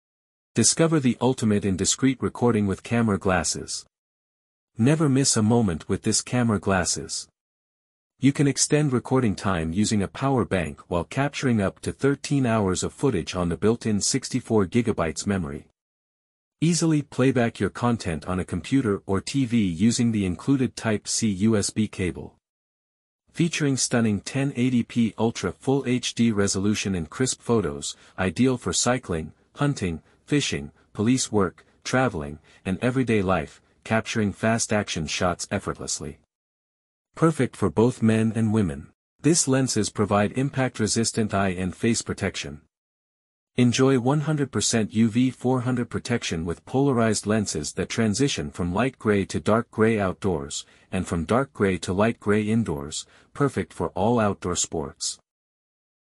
Amazon gadget #7 || 4k camera glasses
Amazon's 4K Camera Glasses redefine wearable technology, blending high-resolution video recording capabilities with stylish eyewear. These innovative glasses allow users to effortlessly capture life's moments in stunning detail, hands-free. Perfect for adventurers, content creators, and anyone looking to document experiences without cumbersome equipment, these glasses offer seamless integration with Amazon's ecosystem for easy sharing and management of footage. Whether for capturing action-packed adventures or everyday memories, Amazon's 4K Camera Glasses deliver unmatched convenience and quality.

 4k camera glasses